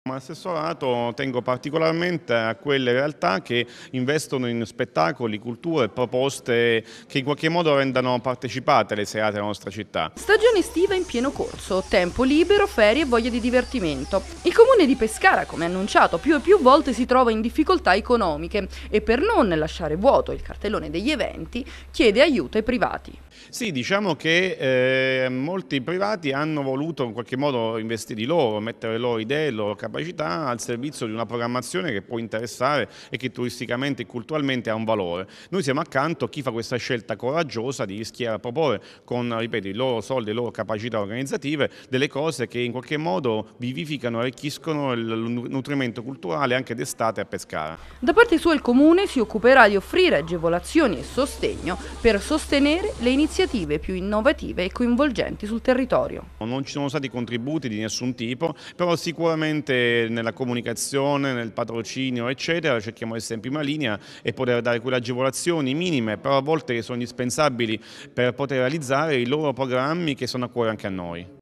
Di Iacovo L'Estate degli spettacoli in spiaggia
Spettacoli sulla spiaggia, cooking show e sport, il comune di Pescara presenta la formula del cartellone estivo con il sostegno dei privati.